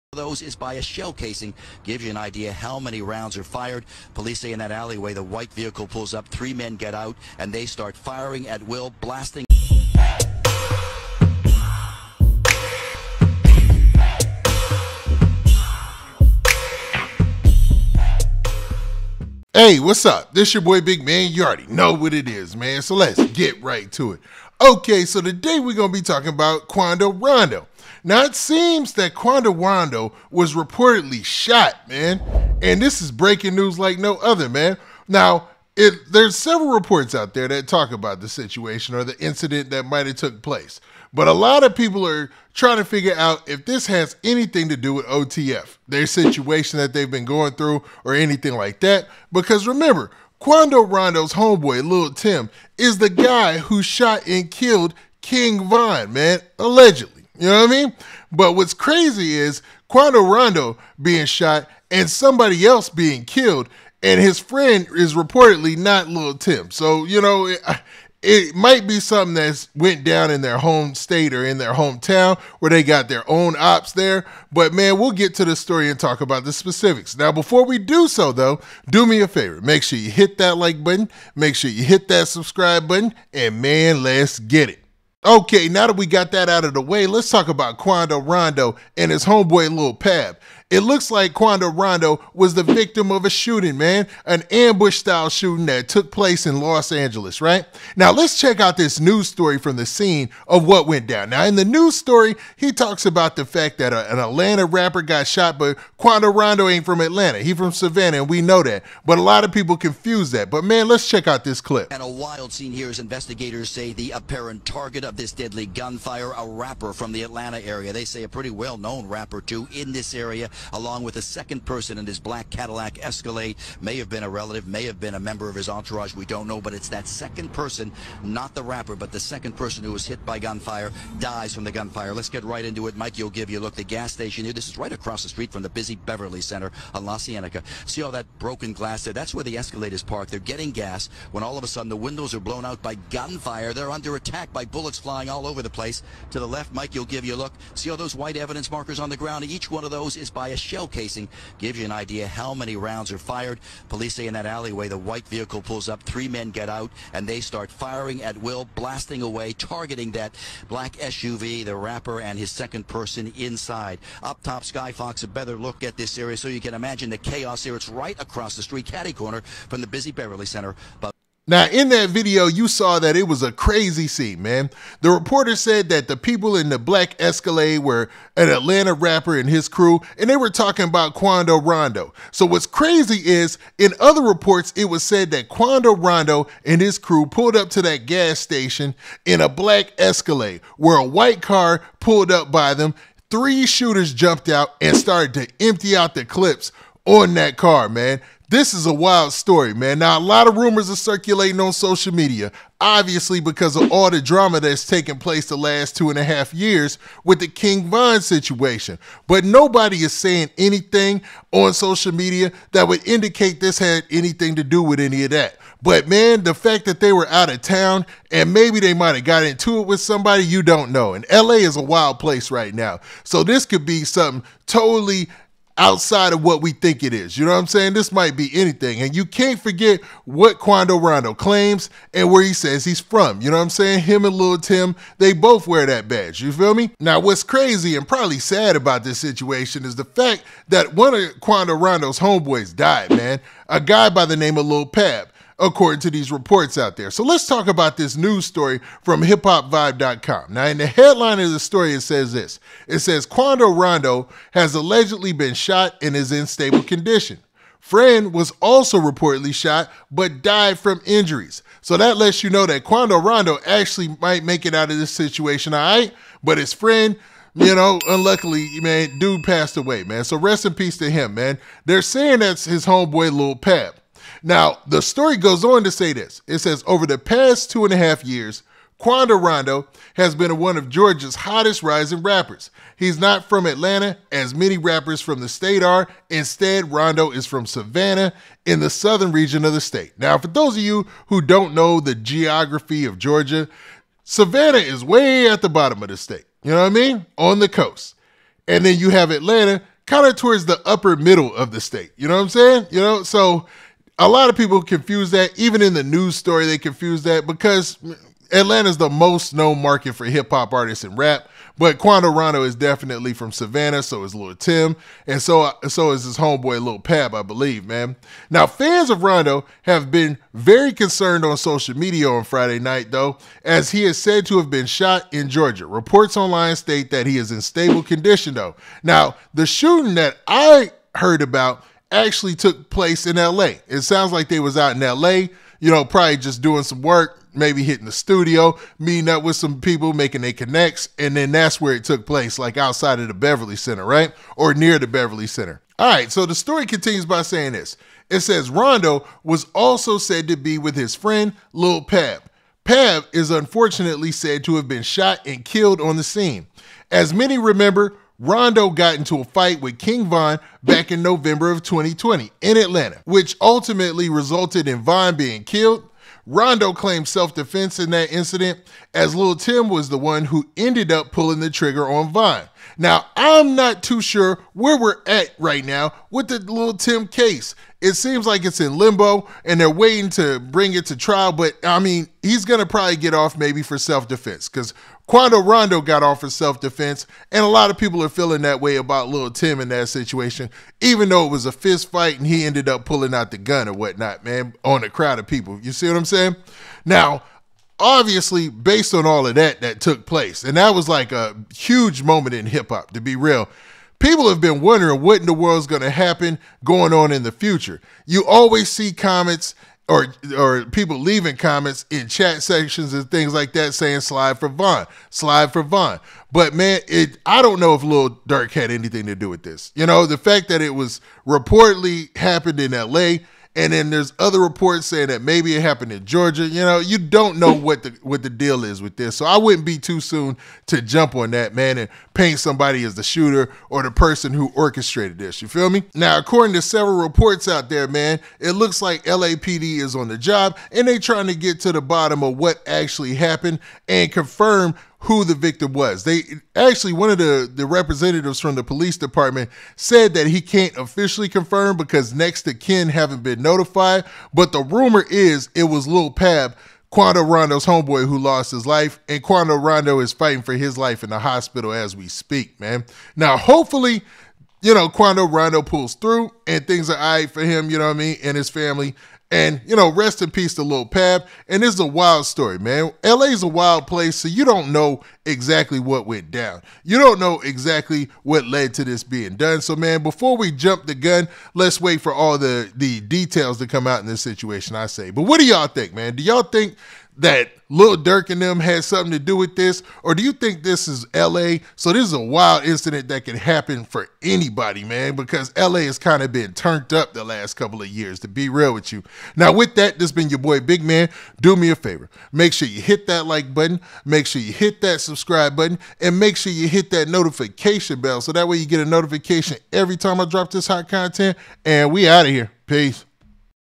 Breaking News!! Quando Rondo Shot & His Friend Dead According to Reports!!
Rapper Quando Rondo was reportedly shot and a friend reportedly passed away in the same shooting!!